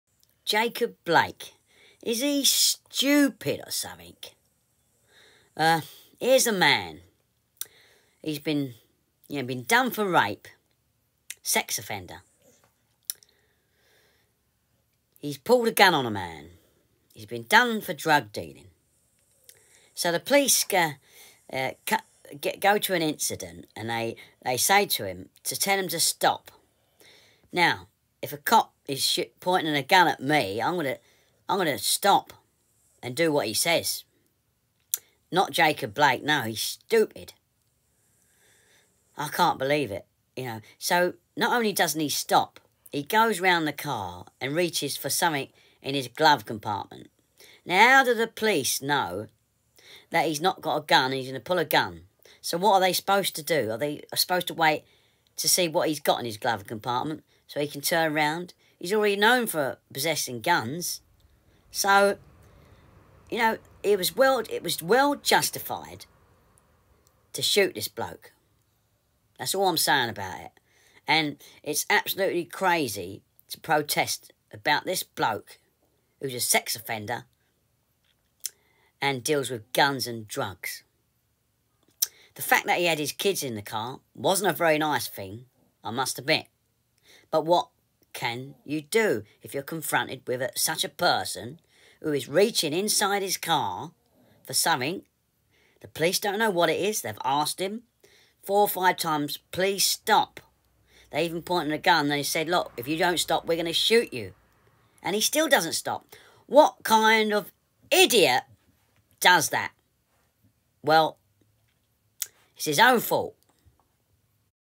Jacob Blake stupid or what!!
He has been told by police officers to stop and he doesn’t ..unbelievable!!!